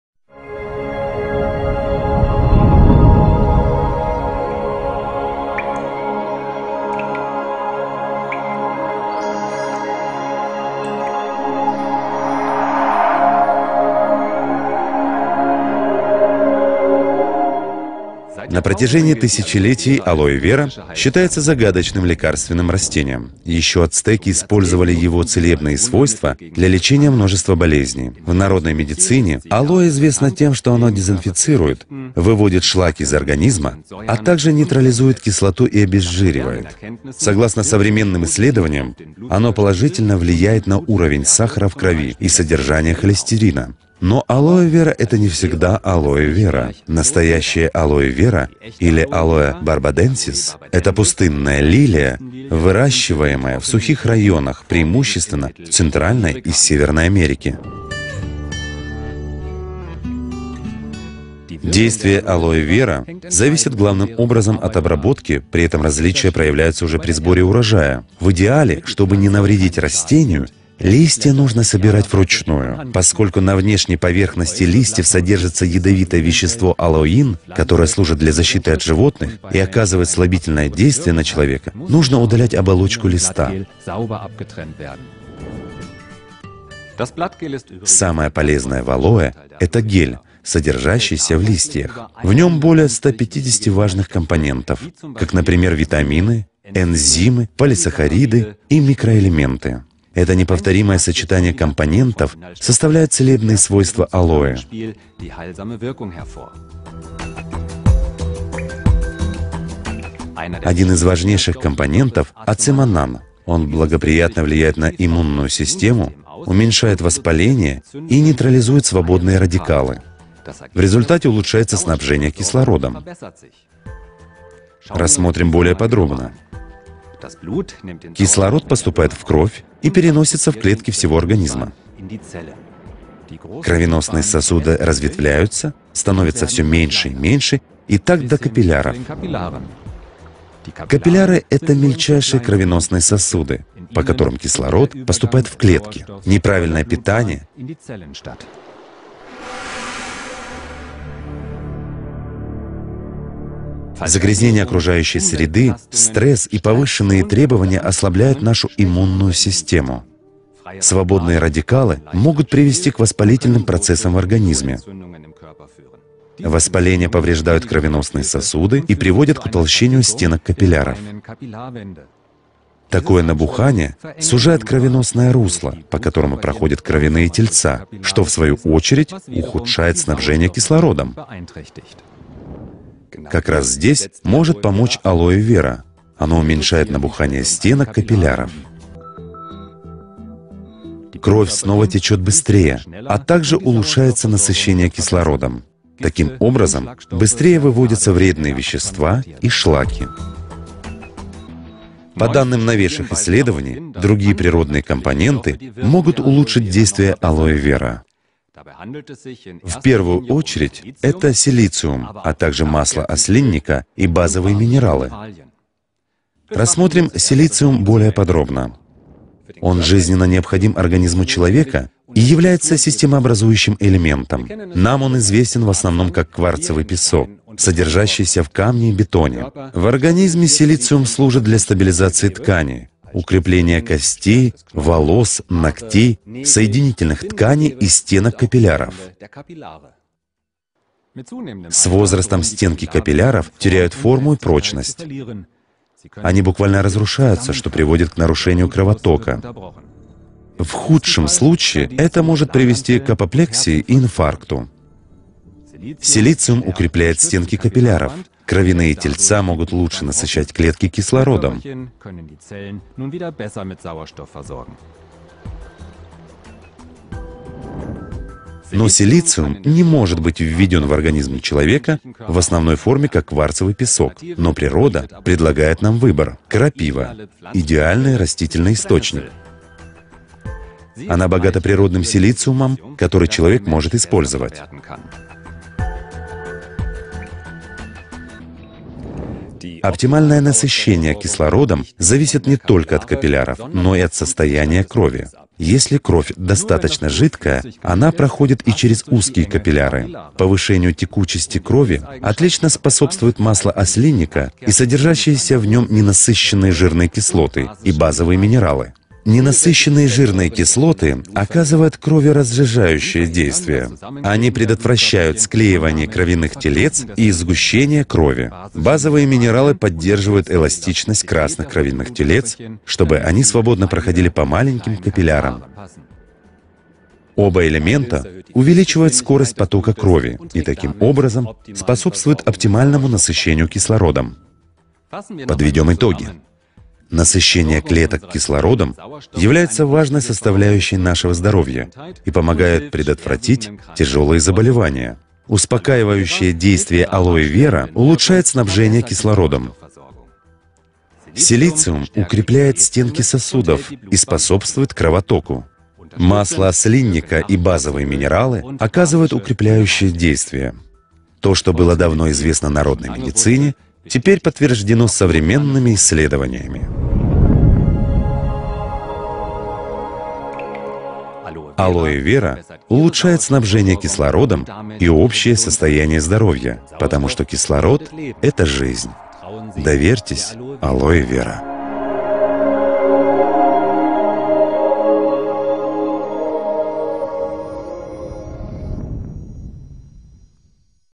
Алоэ Вера. Ролик на русском от LR
Алоэ Вера -"тихий лекарь". Узнайте, какие свойства Алоэ Вера наиболее ценны для человека и животных?